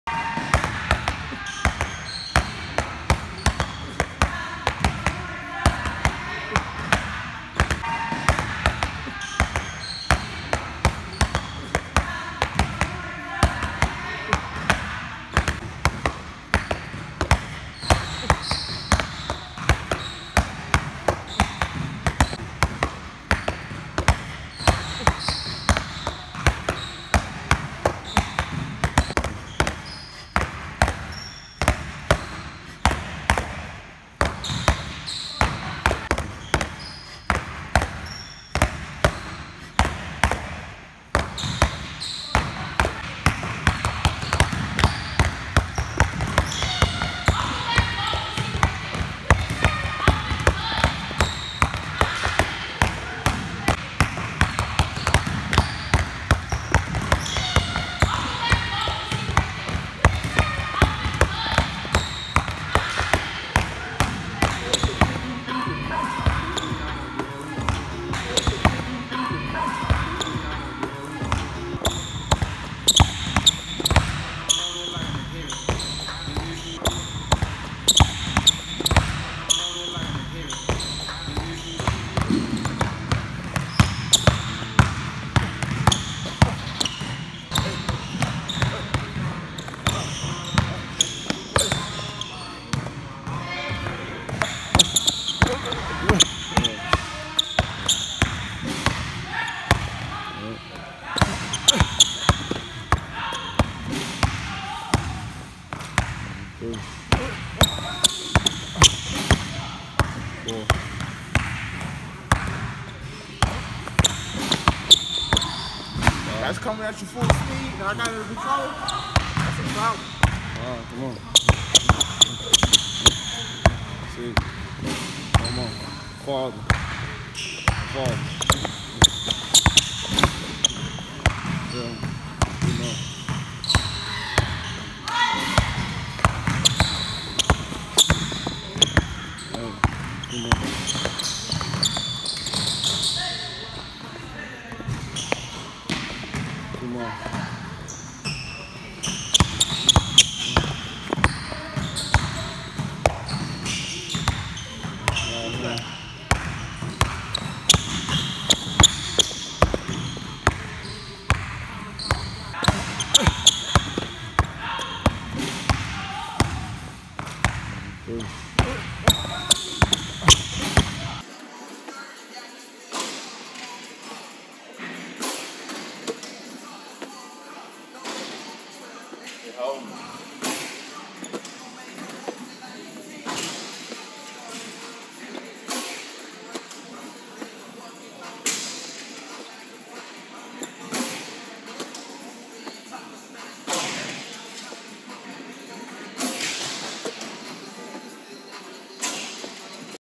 Basketball workout, heavy ball, Vertimax, Dr. Dish shooting, quick moves to layups and jump shots
Steady working 🏀